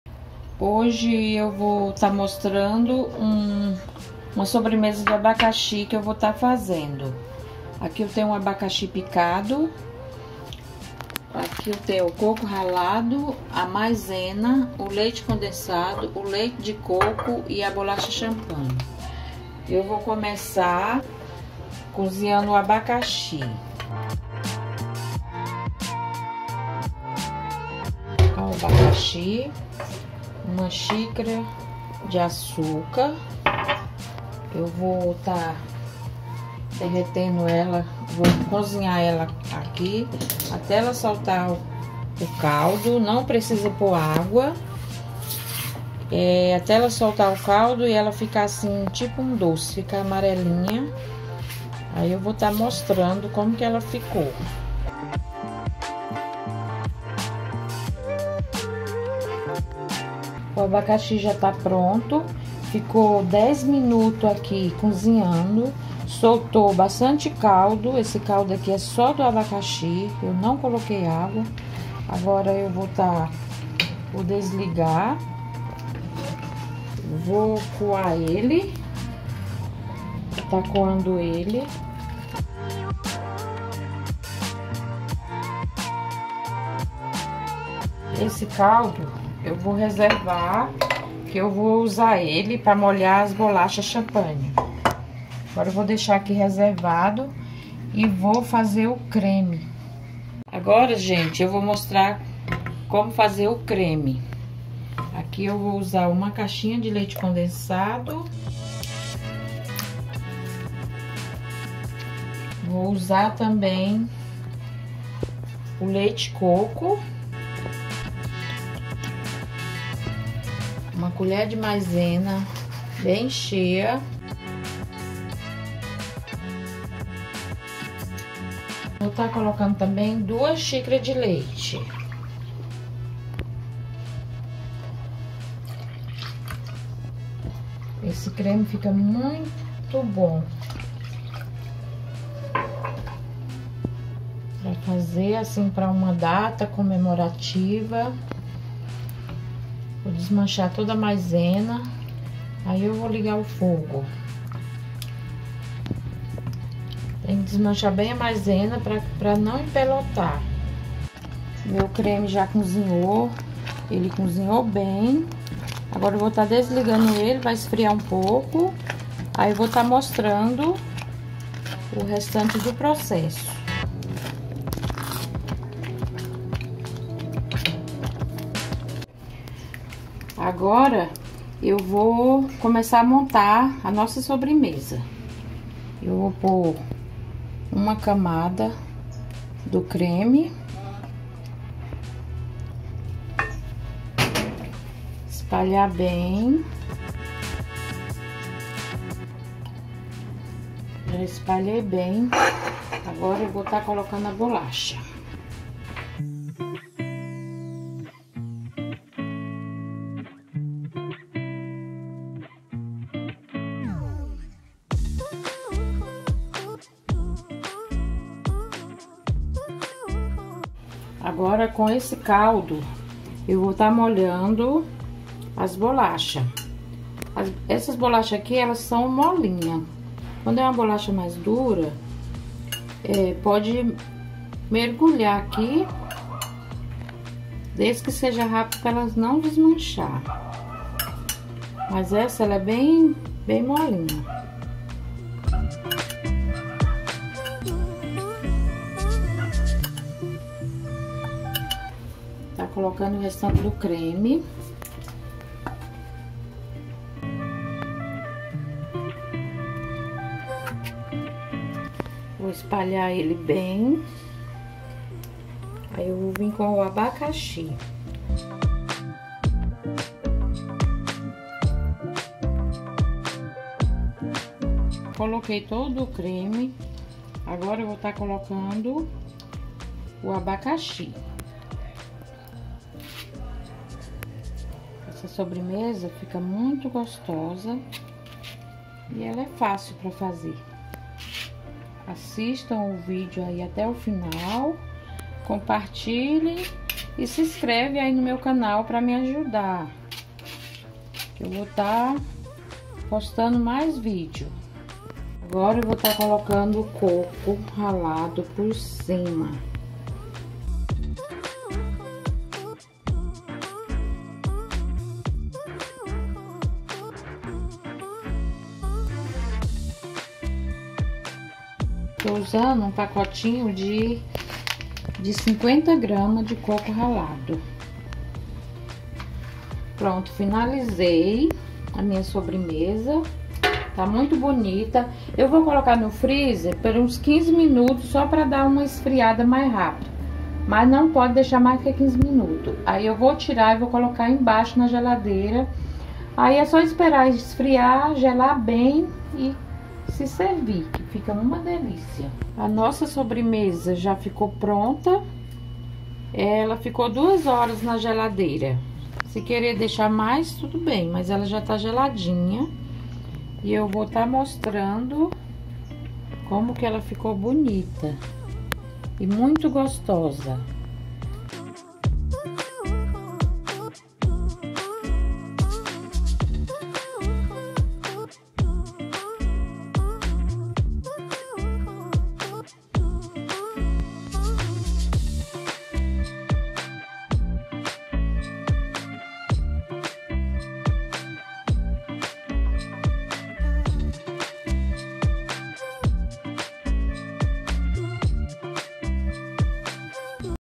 SOBREMESA INCRÍVEL | DELICIA DE ABACAXI COM COCO!!
hoje nesse video vou ensinar a como fazer a sobremesa delicia de abacaxi,uma receita simples e muito gostosa.receita de sobremesa de abacaxi com biscoito, leite condensado e creme de leite.